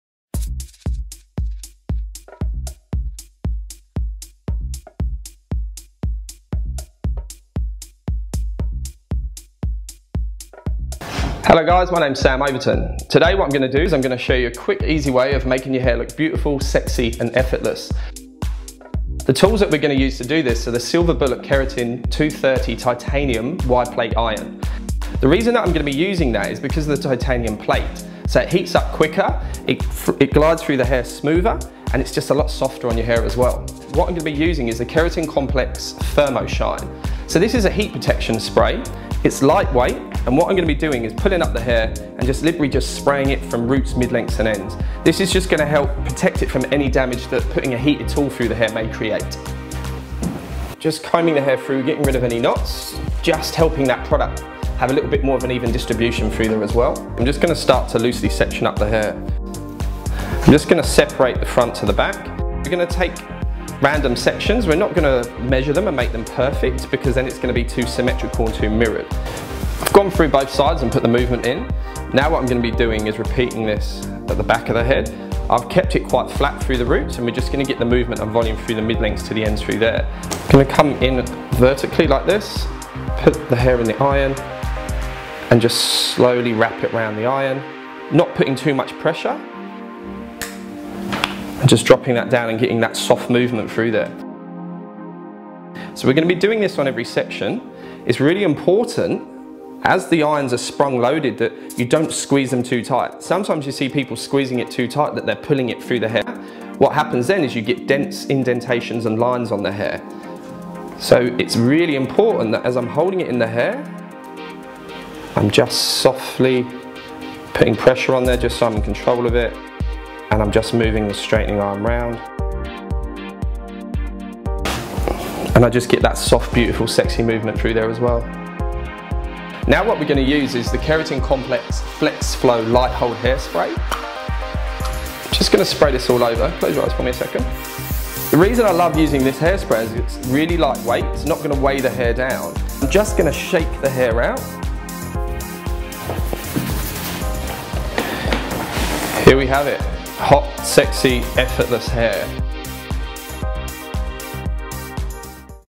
Silver Bullet How To - Effortless Waves - i-glamour.com
Want to create effortless waves? Silver Bullet shows you how.
- OR FOR A LOOSER MOVEMENT TRY THE NEW, Silver Bullet Keratin 230 Titanium Wide Plate Straightener
- Silver Bullet Paddle Brush (free with straightener)
- Crocodile Clips (free with straightener)